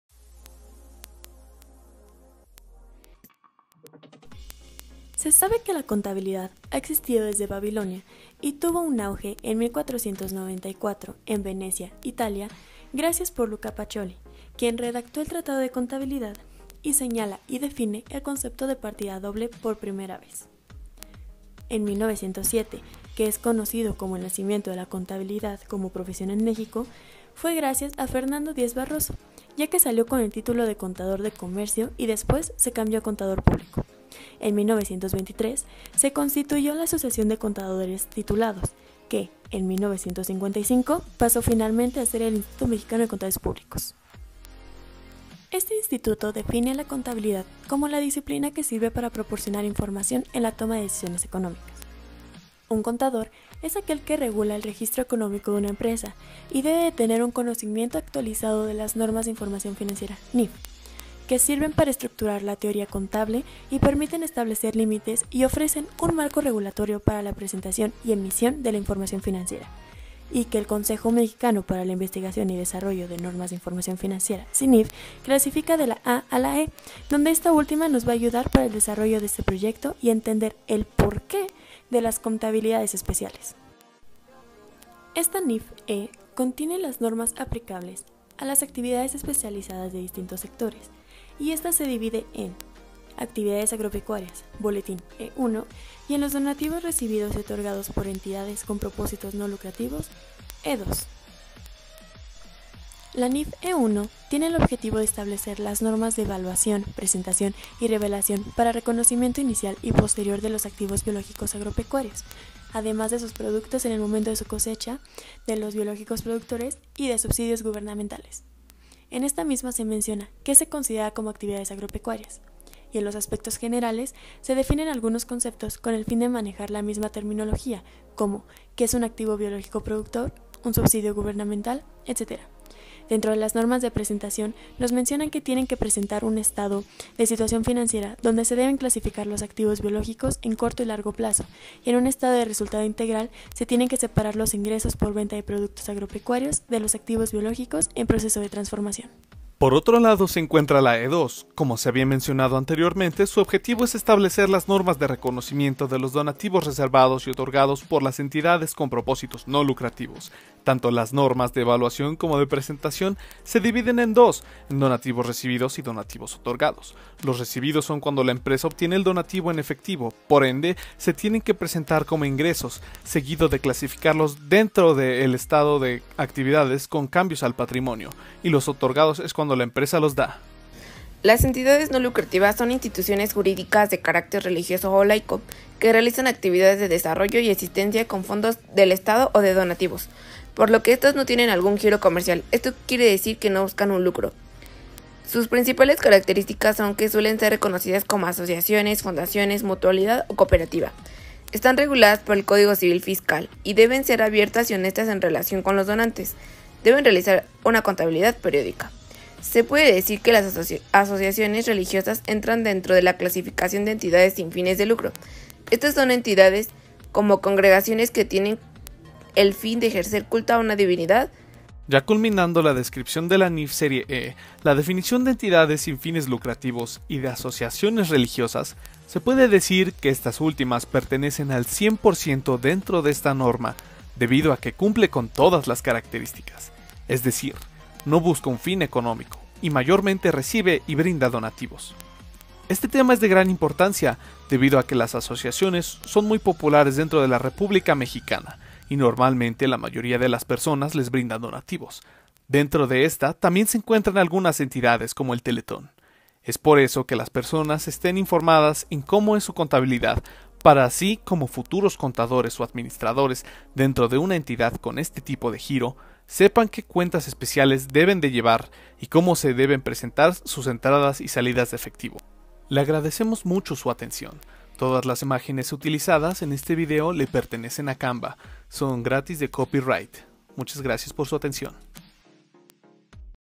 Asociaciones religiosas Normas de Información Financiera serie E
Este VideoEnsayo explica la NIF serie E y las características de las entidades sin fines de lucro, un trabajo elaborado por Daniela Gutiérrez Moreno, María Isabel Parra Carrillo y Sebastián Archundia Ramírez, Alumnos de la Universidad Intercontinental - Campus Sur.